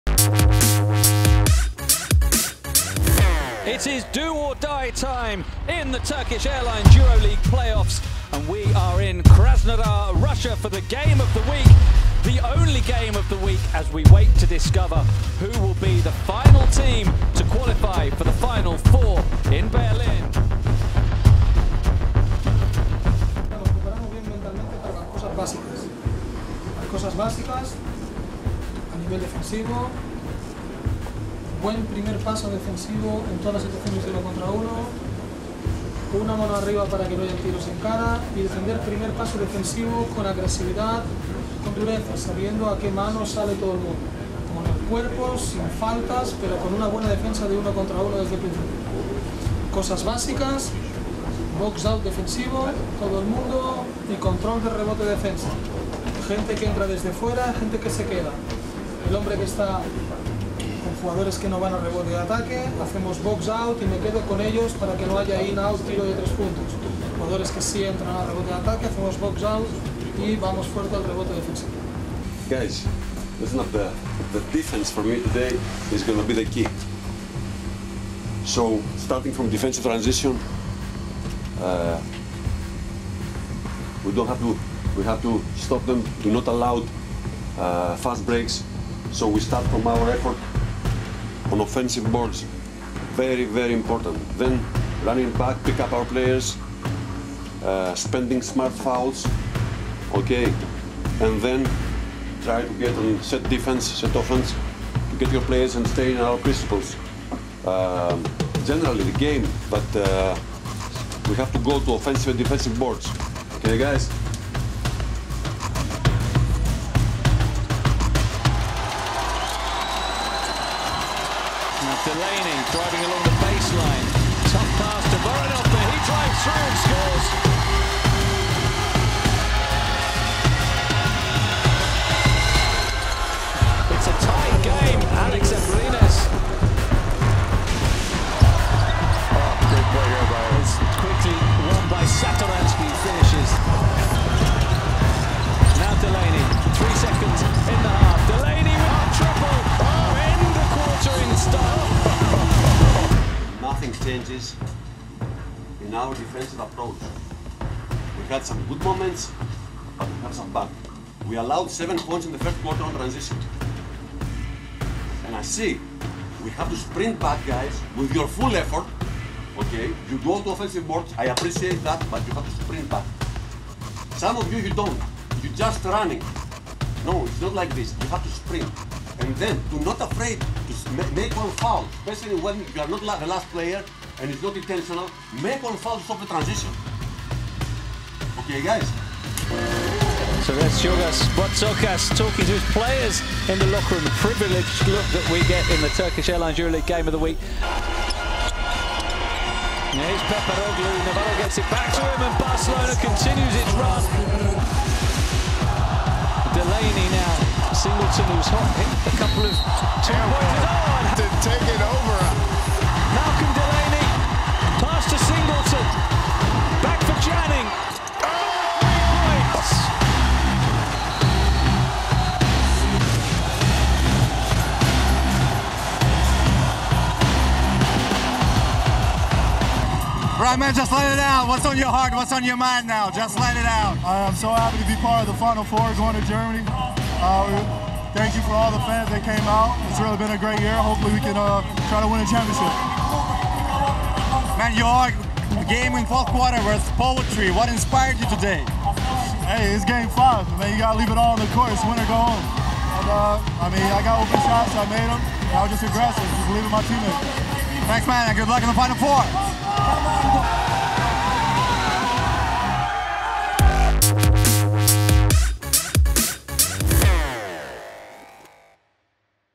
Inside Lokomotiv Kuban Krasnodar - FC Barcelona Lassa
Re-live the best moments of the Playoffs Game 5 between Lokomotiv Kuban Krasnodar and FC Barcelona Lassa from the inside! InsideEuroleague.